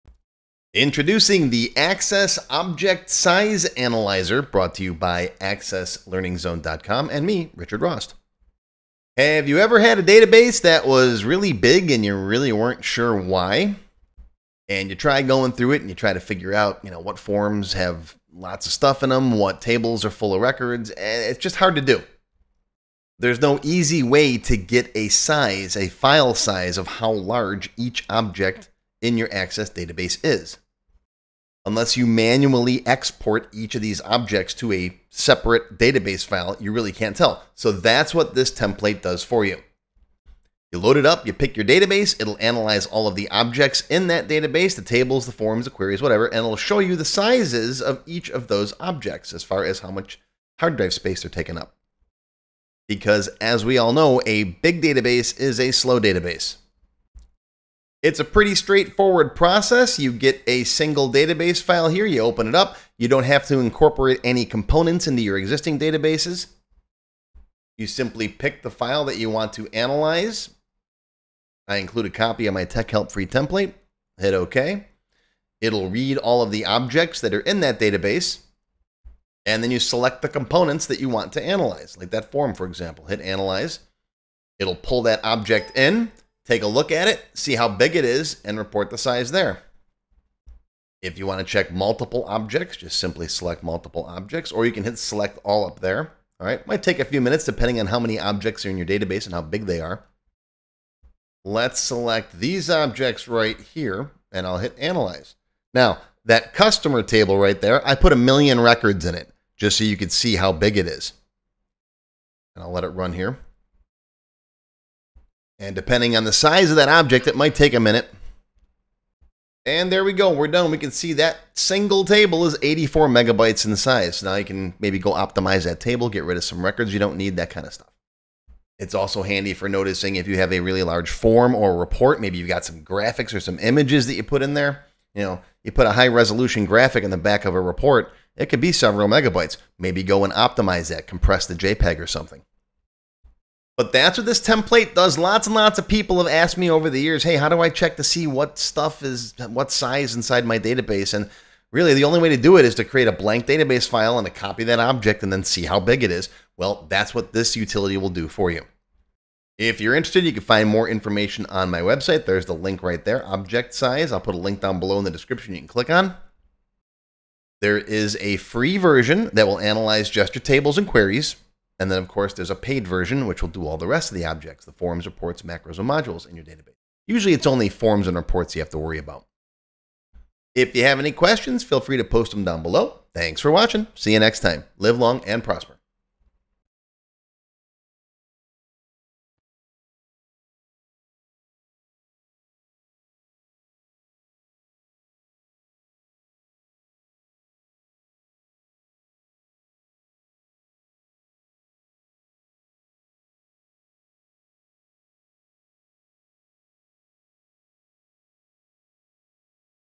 Microsoft Access Object Size Analyzer - Determine Sizes of Objects in Your Database
Do you have a database that is unusually large and you can't figure out why? The Microsoft Access Object Size Analyzer by Access Learning Zone will allow you to quickly and easily see the sizes of all of the individual objects (tables, forms, reports, queries, macros, and modules) in your Microsoft Access database files.

Have you ever had a database that was really big and you really weren't sure why? And you try going through it and you try to figure out what forms have lots of stuff in them, what tables are full of records, it's just hard to do. way to get a size, a file size of how large each object in your Access database is. Unless you manually export each of these objects to a separate database file, you really can't tell. So that's what this template does for you. You load it up, you pick your database, it'll analyze all of the objects in that database, the tables, the forms, the queries, whatever, and it'll show you the sizes of each of those objects as far as how much hard drive space are taken up.

Because as we all know, a big database is a slow database. It's a pretty straightforward process. You get a single database file here. You open it up. You don't have to incorporate any components into your existing databases. You simply pick the file that you want to analyze. I include a copy of my Tech Help 3 template, hit OK. It'll read all of the objects that are in that database and then you select the components that you want to analyze, like that form for example. Hit analyze. It'll pull that object in, take a look at it, see how big it is and report the size there. If you want to check multiple objects, just simply select multiple objects or you can hit select all up there. It might take a few minutes depending on how many objects are in your database and how big they are.

Let's select these objects right here and I'll hit analyze. Now that customer table right there, I put a million records in it just so you could see how big it is. I'll let it run here. And depending on the size of that object, it might take a minute. And there we go, we're done. We can see that single table is 84 megabytes in size. Now you can maybe go optimize that table get rid of some records you don't need that kind of stuff. It's also handy for noticing if you have a really large form or report maybe you've got some graphics or some images that you put in there you know you put a high-resolution graphic in the back of a report it could be several megabytes maybe go and optimize that compress the JPEG or something. But that's what this template does lots and lots of people have asked me over the years, hey, how do I check to see what size inside my database?

And really, the only way to do it is to create a blank database file and to copy that object and then see how big it is. Well, that's what this utility will do for you. If you're interested, you can find more information on my website. There's the link right there, object size. I'll put a link down below in the description you can click on. There is a free version that will analyze gesture tables and queries.

And then of course there's a paid version which will do all the rest of the objects, the forms, reports, macros, and modules in your database. Usually it's only forms and reports you have to worry about. If you have any questions, feel free to post them down below. Live long and prosper.